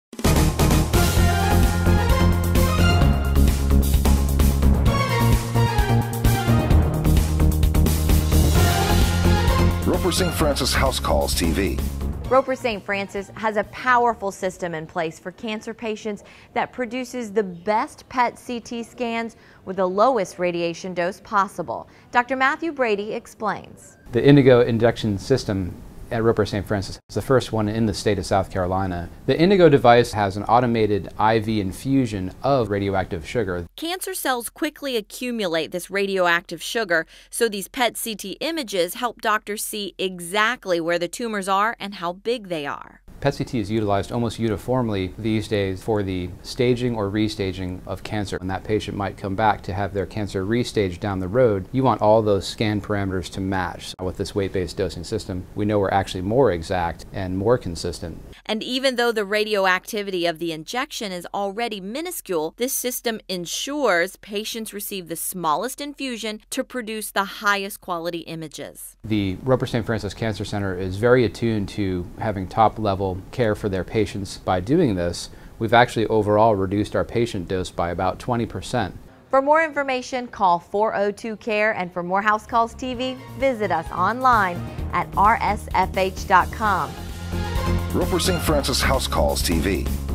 PET/CT Intego Low Dose Injection System
Roper St. Francis has a powerful system in place for cancer patients that produces the best PET/CT scans with the lowest radiation dose possible. Dr. Matthew Brady explains.